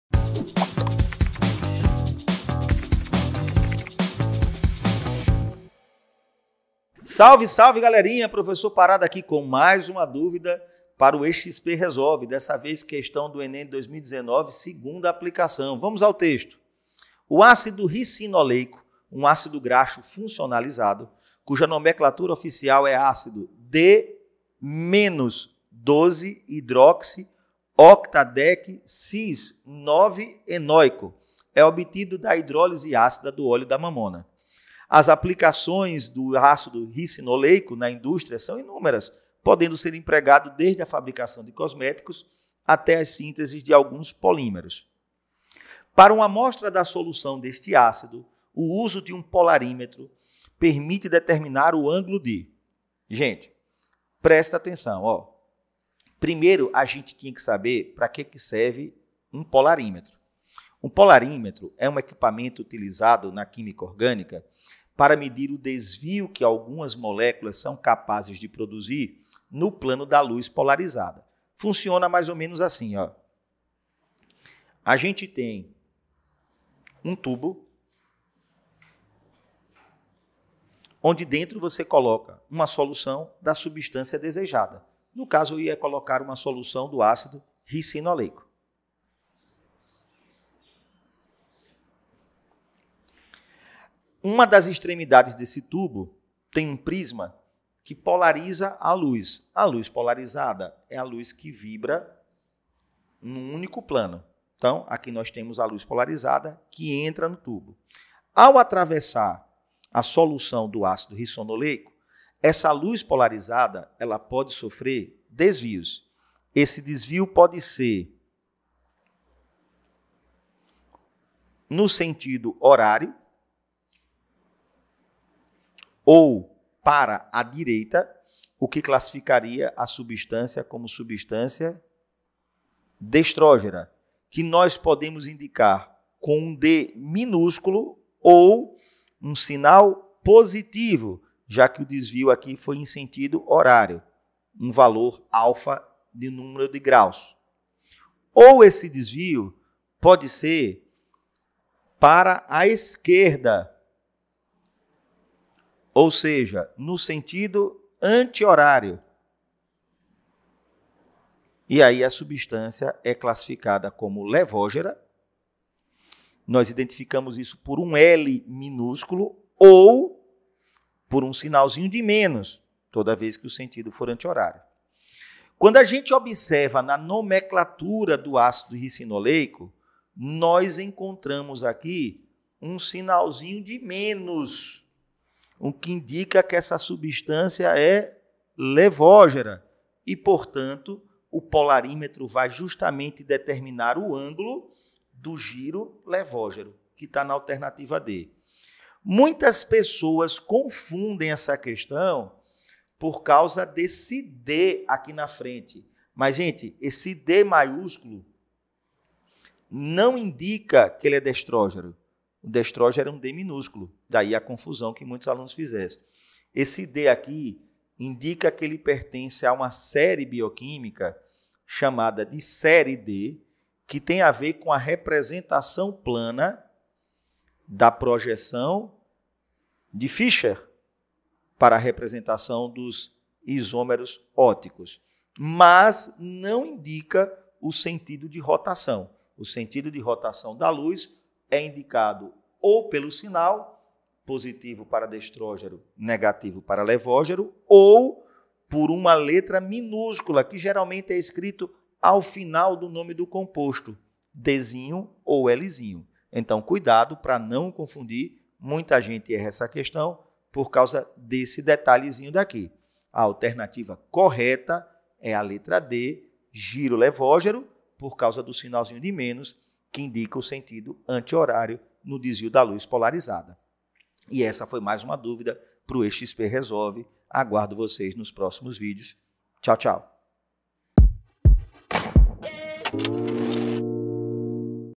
O ÁCIDO RICINOLEICO, UM ÁCIDO GRAXO FUNCIONALIZADO, CUJA NOMENCLATURA (...) | ISOMERIA B ESPACIAL
Encontre a sua questão nos seguintes cadernos:

NO NOSSO MANUAL DE SOBREVIVÊNCIA:
ASSUNTO: 3 QUÍMICA ORGÂNICA | 3.5 ISOMERIA B. ESPACIAL

QUESTÃO 02
(ENEM 2019 2° APLICAÇÃO) O ácido ricinoleico, um ácido
graxo funcionalizado, cuja nomenclatura ofi cial é ácido D-(−)-
12-hidroxi-octadec-cis-9-enoico, é obtido da hidrólise ácida do
óleo de mamona. As aplicações do ácido ricinoleico na indústria
são inúmeras, podendo ser empregado desde a fabricação de
cosméticos até a síntese de alguns polímeros.

Para uma amostra de solução desse ácido, o uso de um
polarímetro permite determinar o ângulo de

A refração.
B refl exão.
C difração.
D giro levógiro.
E giro destrógiro.